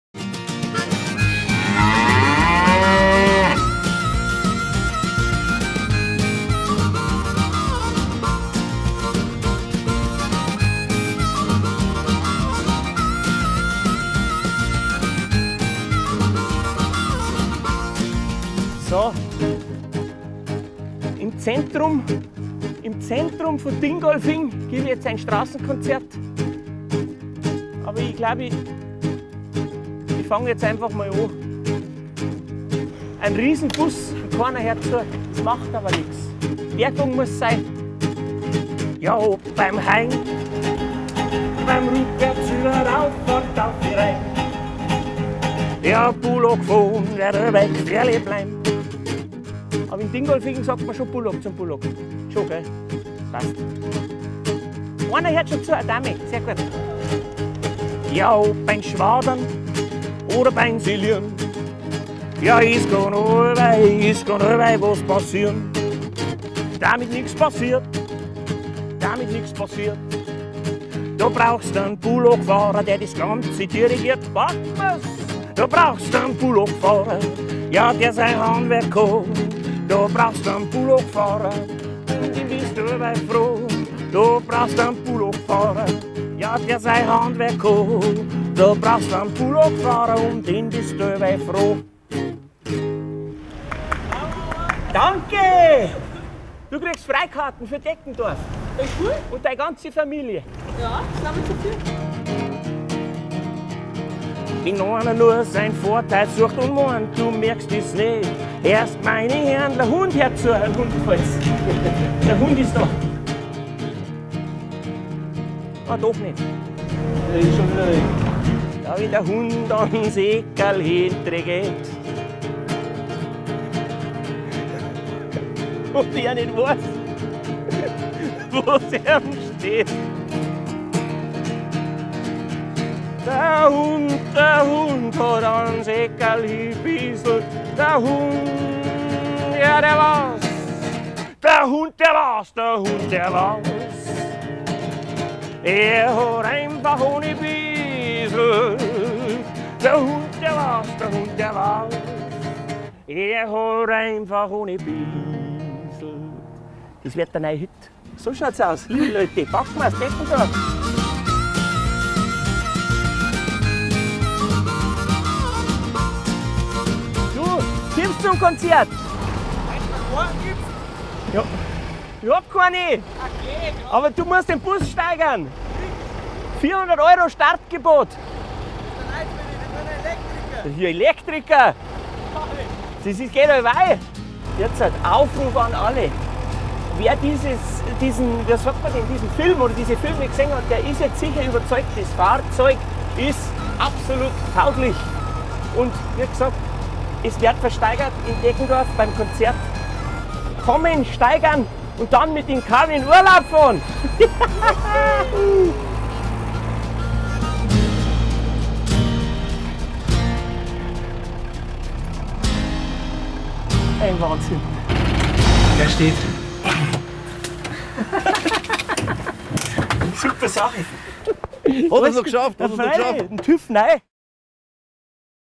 Der letzte musikalische Zwischenstopp - der Keller Steff-Tourbus auf dem Weg nach Deggendorf!
Mit einer ungewöhnlichen Aktion macht der Liedermacher Keller Steff derzeit auf sich aufmerksam: auf einem Konzert am Freitag in Deggendorf will er unter den Konzertbesuchern seinen alten Tourbus versteigern – und zwar für den guten Zweck. Dafür musste der Bus aber erstmal vom Chiemsee nach Deggendorf kommen – und diese letzte Fahrt, die haben wir exklusiv begleiten dürfen! Inklusive eines spontanen, musikalischen Halts im Herzen von Dingolfing…